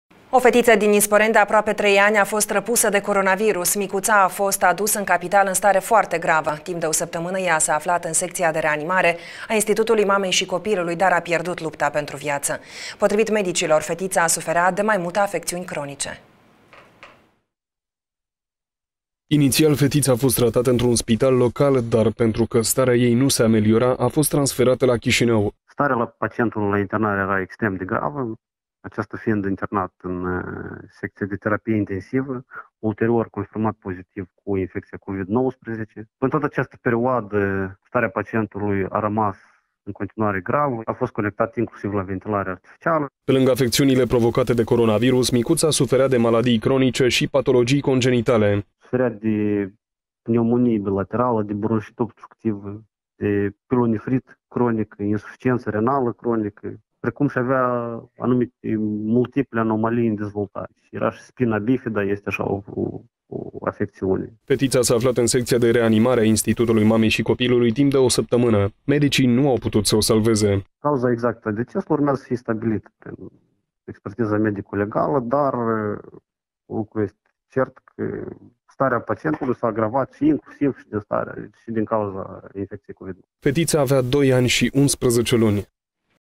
Copil răpus de coronavirus
O fetița din Nisporeni, de aproape 3 ani, a fost răpusă de coronavirus. Micuța a fost a fost adusă în capitală în stare foarte gravă. Timp de o săptămână ea s-a aflat în secția de reanimare a Institutului Mamei și Copilului, dar a pierdut lupta pentru viață. Potrivit medicilor, fetița suferea de mai multe afecțiuni cronice.